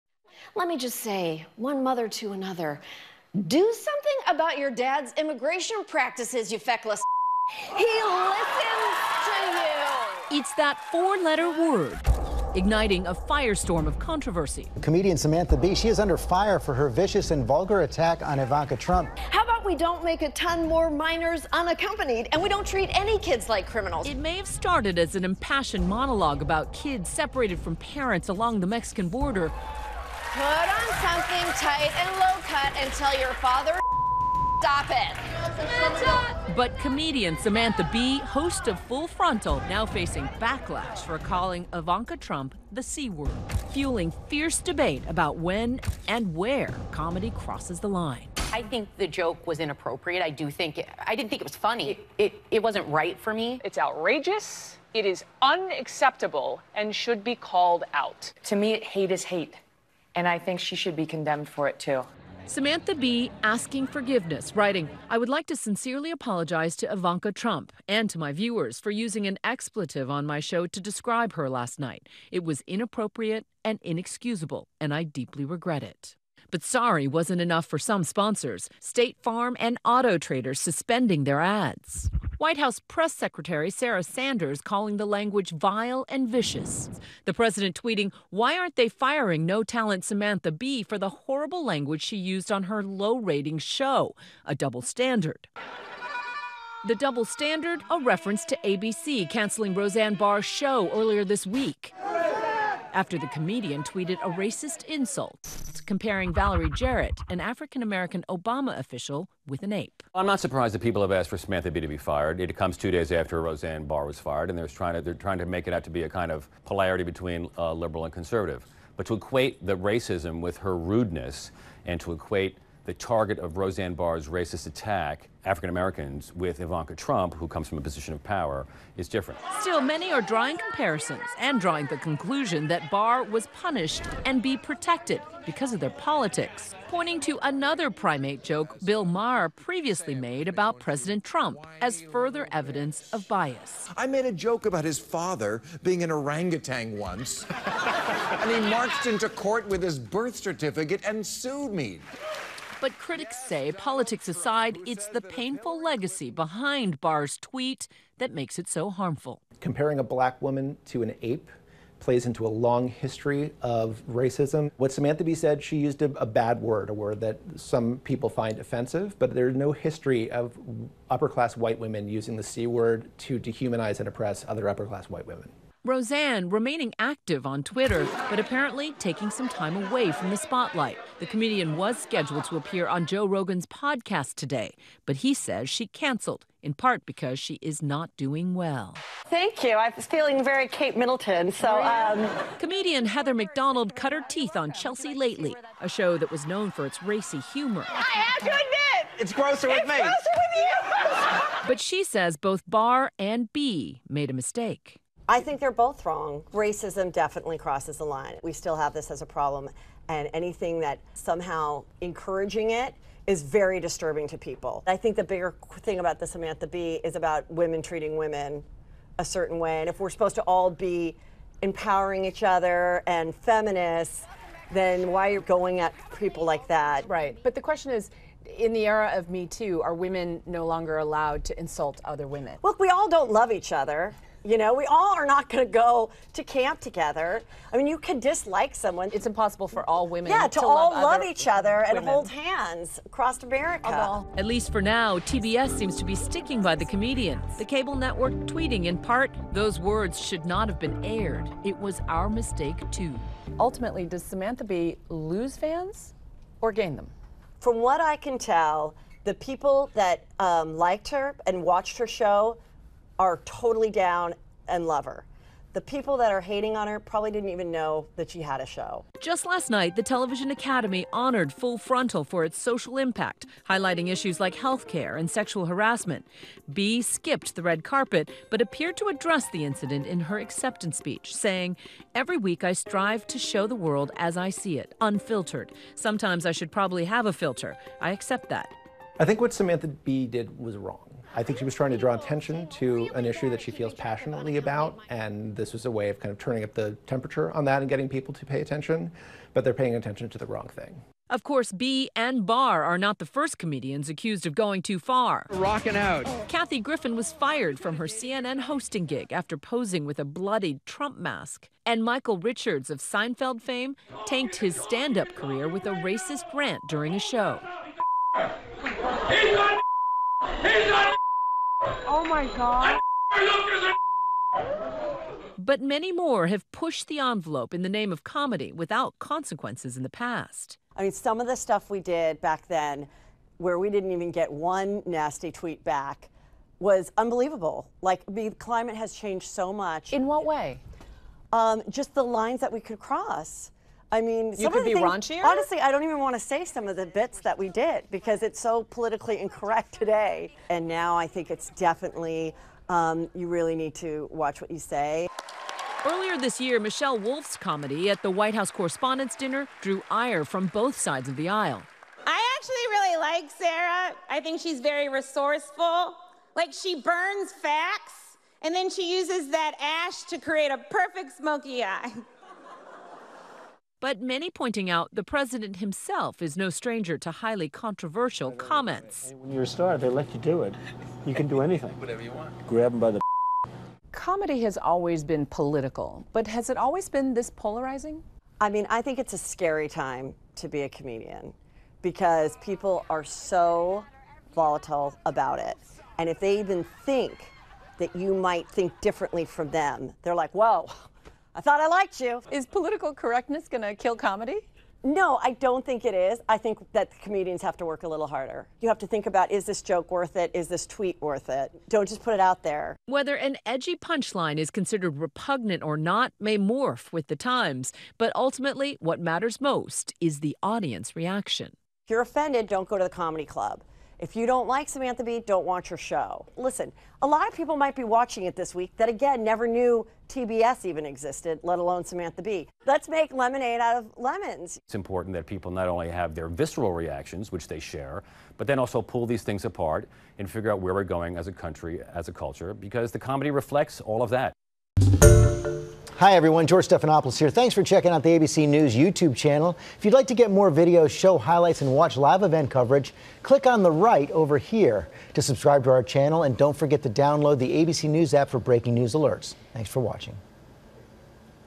The debate over when and where comedy crosses the line
Samantha Bee's calling Ivanka Trump the "c word" on her show "Full Frontal" fueled a fierce debate in the wake of Roseanne Barr's sitcom being canceled after her controversial tweet.